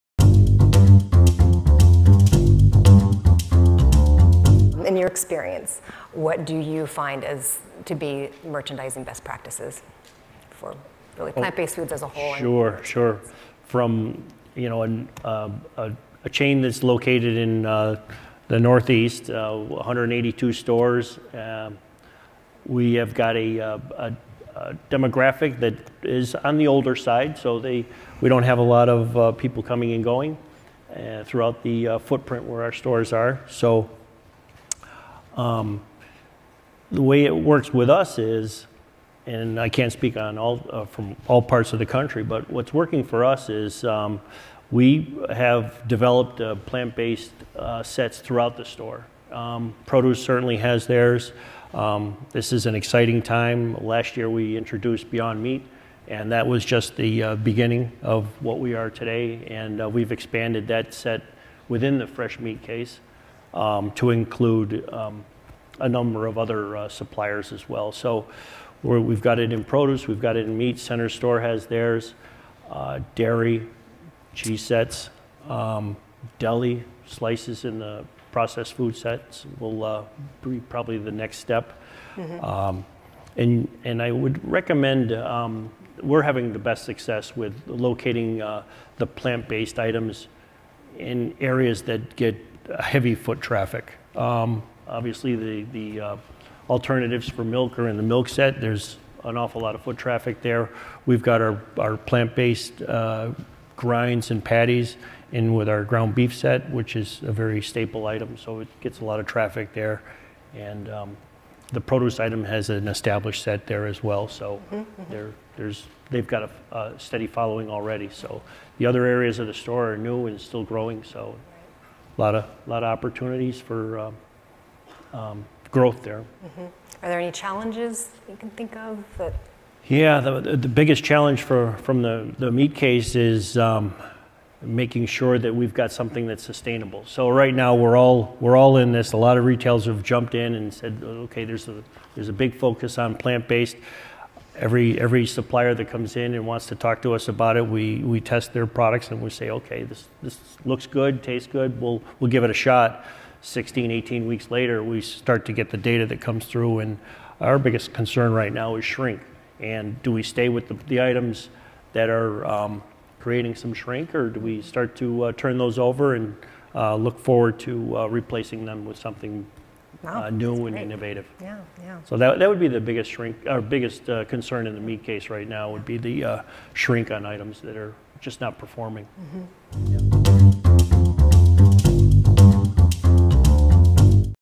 Expanding Your Plant-Based Options in Retail: Merchandising Best Practices
Speaker: Bill Puza, Hannaford Brothers/Ahold Delhaize Company
Winter Fancy Food Show 2020